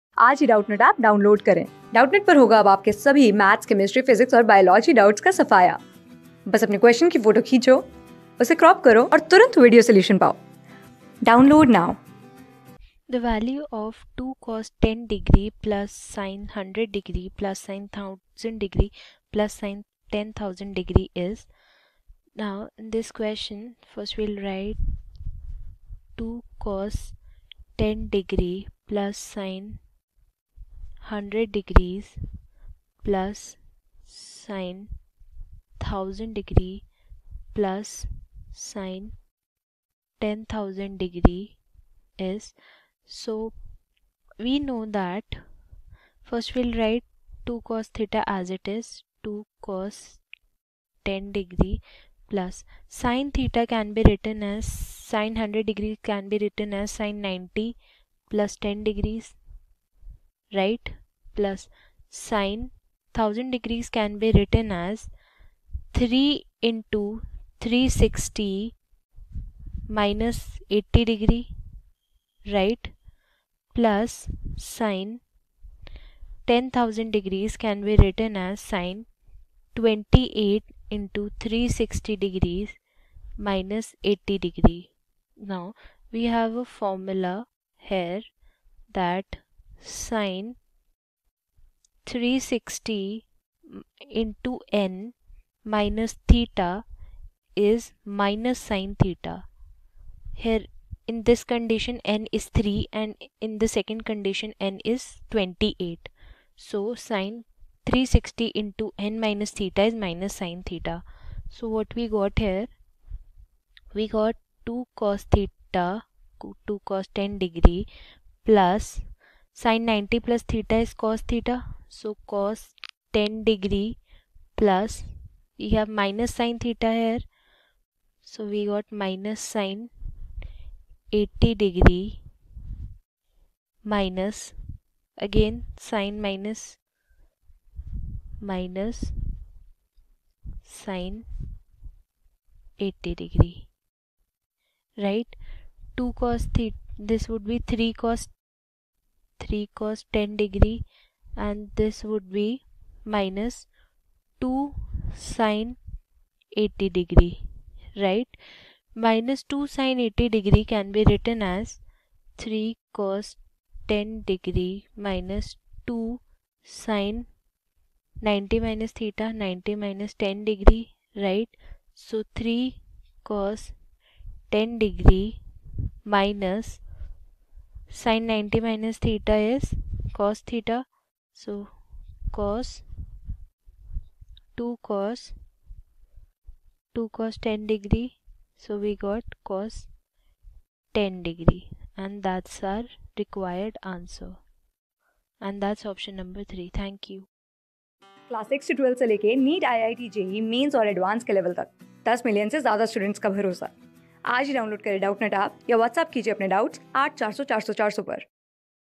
The value of 2 cos10^(@)+sin 100^(@)+sin 1000^(@)+sin 10000^(@) is | CLASS 12 | TRIGONOMETRIC F...
The value of 2 cos10^(@)+sin 100^(@)+sin 1000^(@)+sin 10000^(@) is
Class: 12
Subject: MATHS
Chapter: TRIGONOMETRIC FUNCTIONS
Board:IIT JEE

👉 Have Any Query? Ask Us.